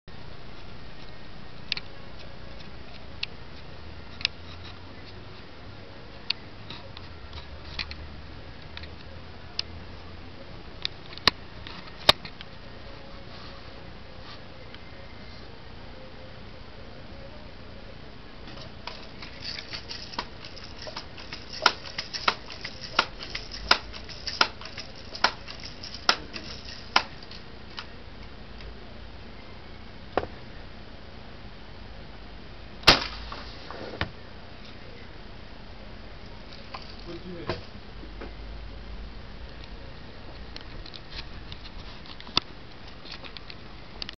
bb-gun shooting pt2
shooting  a hairspray can with bb-gun